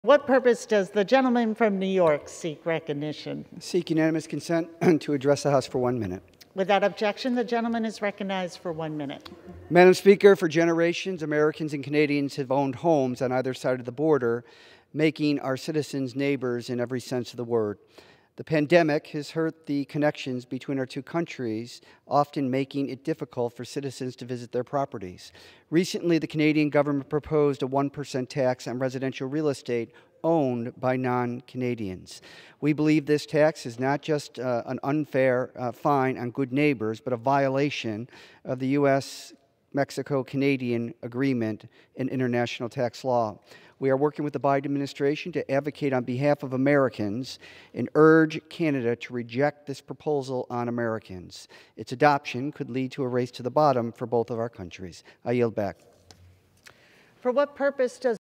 Dem Lawmaker Hammers Canadian Tax Law He Calls A 'Violation' Of The USMCA
In remarks on the House floor Thursday, Rep. Brian Higgins (D-NY) spoke about a new tax in Canada.